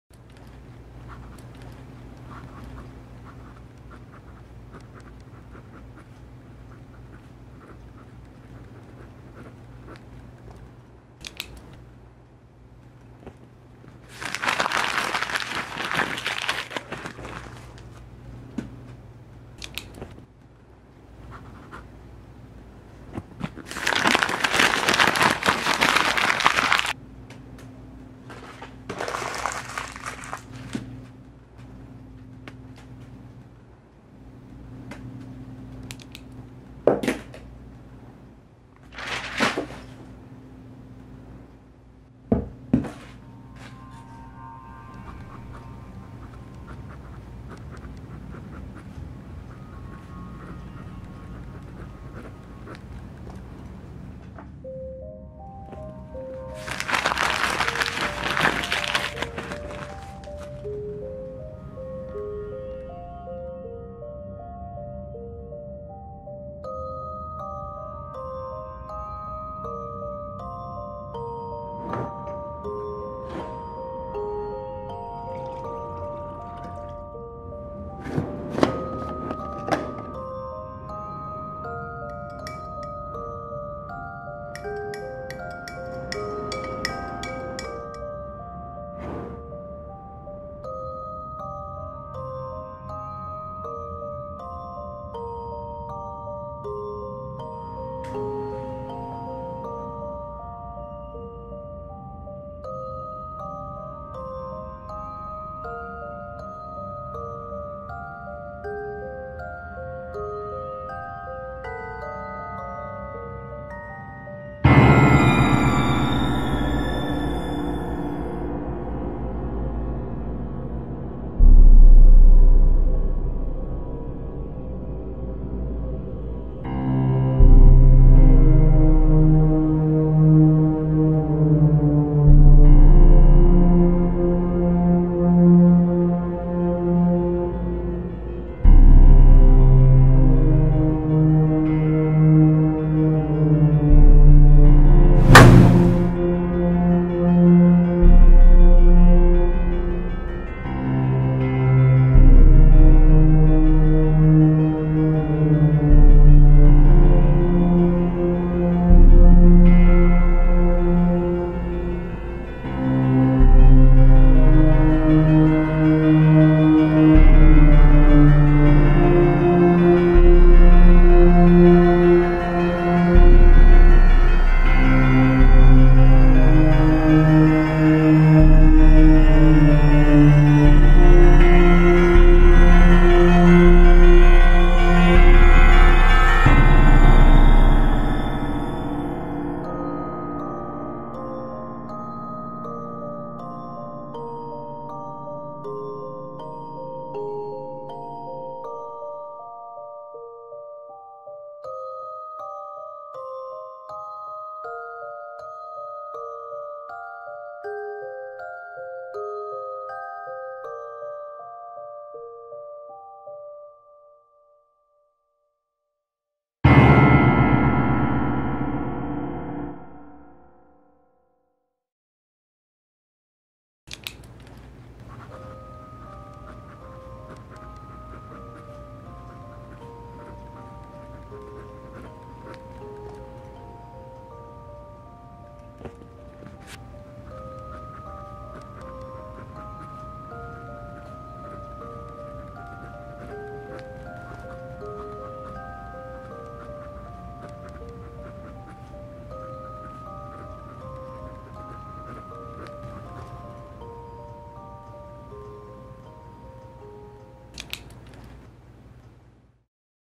How To Peel An Orange (2018)
Peeling an orange is more complicated than it appears... Or so it seems.
Do you know how to peel an orange?

Summer 2018 / Premiere Editing  - Final Project

| Creator